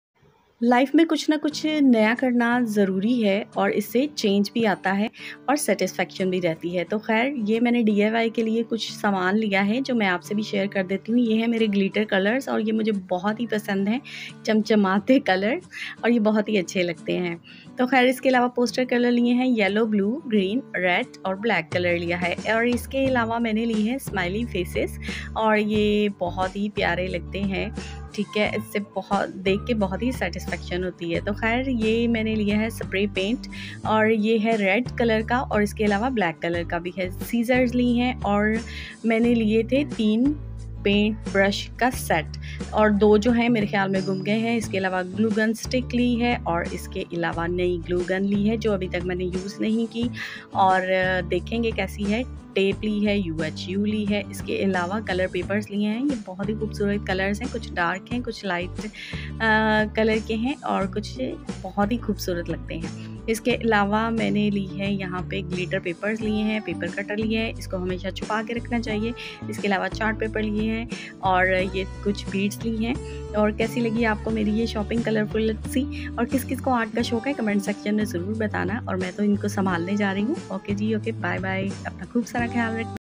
Shopping For Channel & Diy | Seven Day Vlogs By Sam
Hi everyone today I share with you are my small shopping for d i y and shopping for my channel hope you like my videos thanks for your great support please like share and subscribe my channel |Seven Day Vlogs By Sam